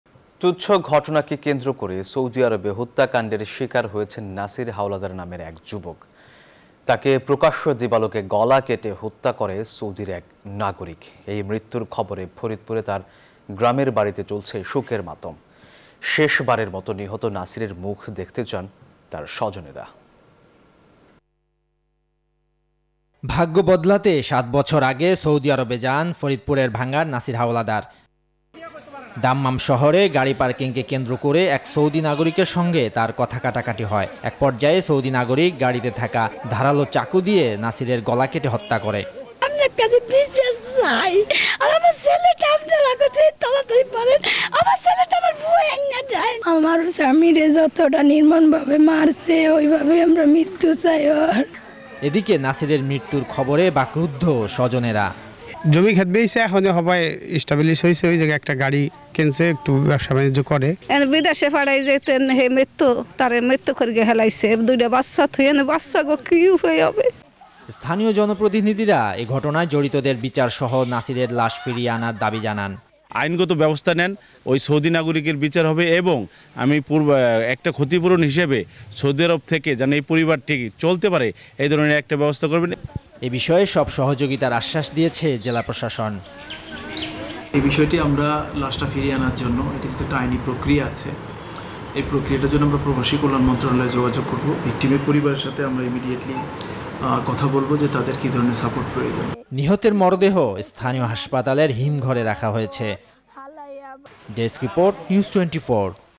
নাসির হাওলাদারের বাড়িতে শোকের মাতম | Faridpur | Saudi Arabia | News24
তুচ্ছ ঘটনাকে কেন্দ্র করে সৌদি আরবে হত্যাকান্ডের শিকার হয়েছেন নাসির হাওলাদার নামের এক যুবক। তাকে প্রকাশ্য দিবালোকে গলাকেটে হত্যা করে সৌদির এক নাগরিক। এই মৃত্যুর খবরে ফরিদপুরে তার গ্রামের বাড়িতে চলছে শোকের মাতম। শেষ বারের মতো নিহত নাসিরের মুখ দেখতে চান তার স্বজনেরা

NEWS24 Team